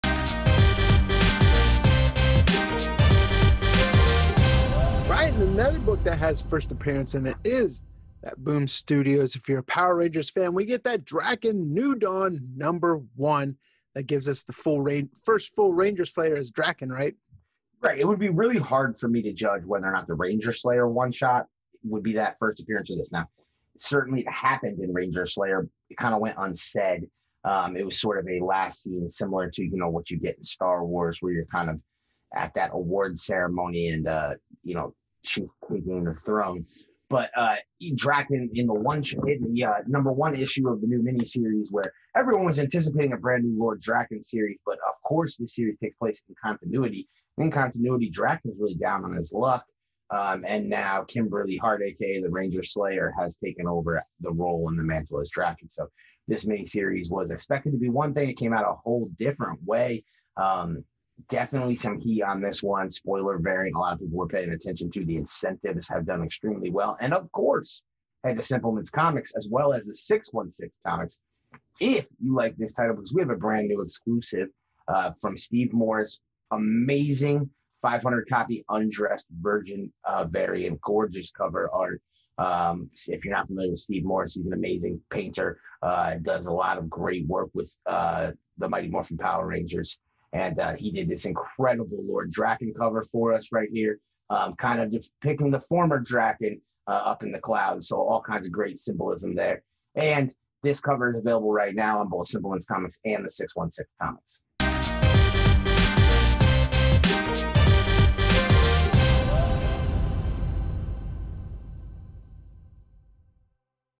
Best Comics of 2020: Drakkon New Dawn #1
After the shocking events of Power Rangers: Ranger Slayer #1, Kimberly is determined to purge the terrifying hold that Drakkon has had over her world, starting with Deadlock; the prison tower where Drakkon held all those who opposed him. But even the Ranger Slayer has no clue of the true power kept in Deadlock—and what unlocking the doors will set into motion. As Kimberly investigates the worst of Drakkon’s secrets, she sets in motion Drakkon’s final plan—one that will pit her against a fan-favorite Power Rangers villain like you’ve never seen them before!